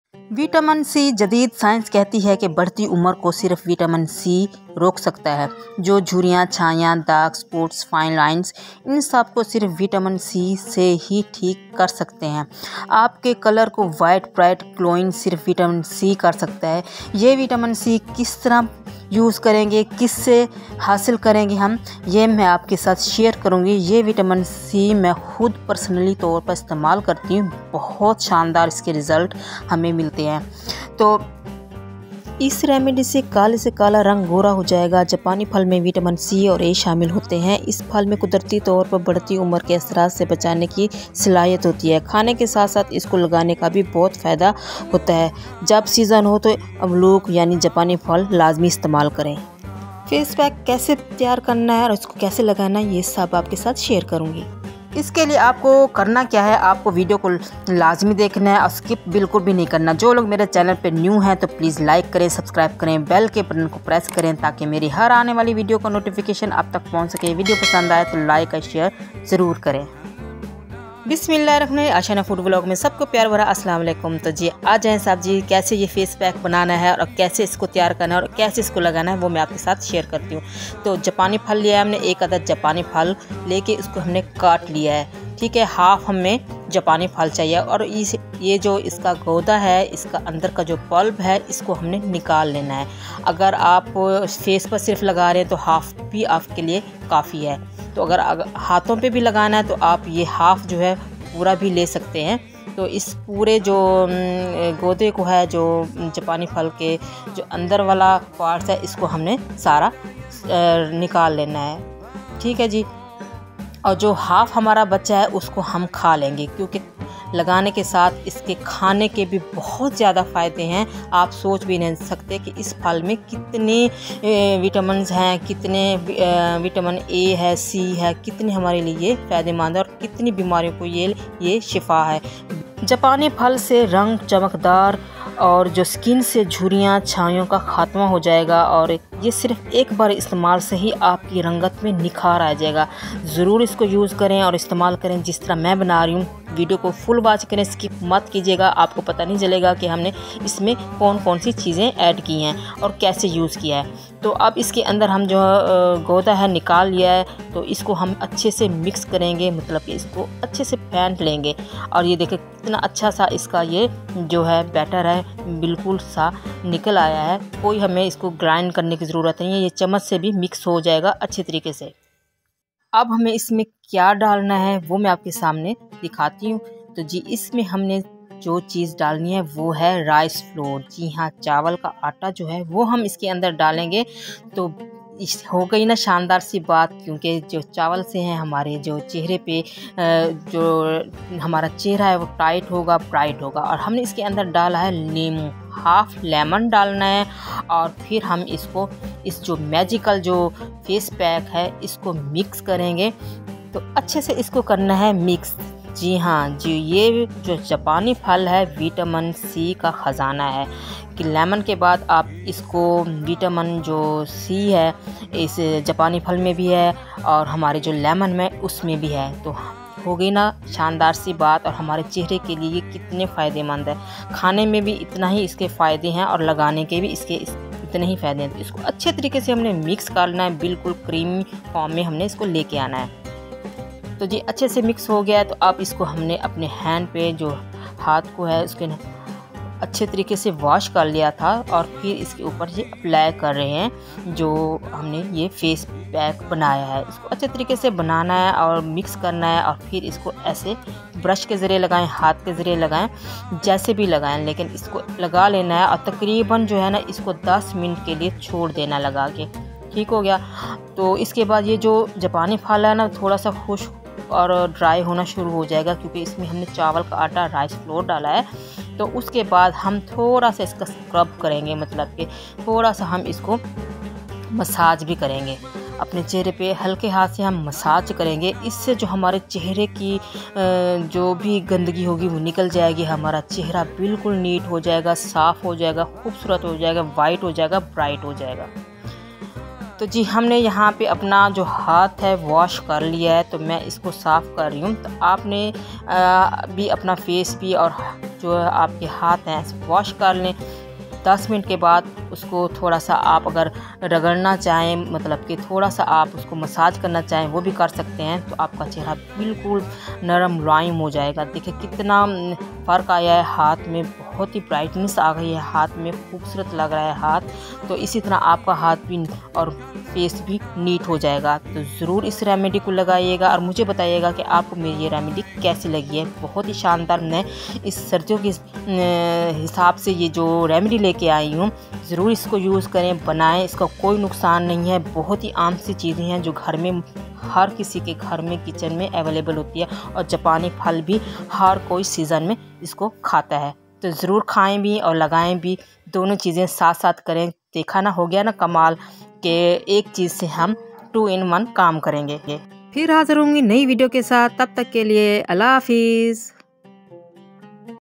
Japani Phal. Amlok Face Pack Mask | Benefits of Persimmons | Japani Phal k Faidy| Skin Whitening
aslamulikum everyone all my you tube friends. I am sharing today benefits of persimmon fruit (Japani Phal,

and also sharing my secret Anti-Aging face mask using persimmon fruit because Persimmons are an excellent source of the potent antioxidant vitamin C. In fact, one persimmon contains 20% of the recommended daily intake and weekly application on skin. Vitamin C helps protect cells from damage caused by free radicals and combats inflammation in the body. Packed with an enormous amount of fiber and antioxidants including Vitamins A, C, B-complex and B-6, beta-carotene, manganese and lycopene, persimmons are considered to be one of nature's super fighters against free radical damage.Remove wrinkles, fine lines and dark spots naturally.
thanks visiting my channel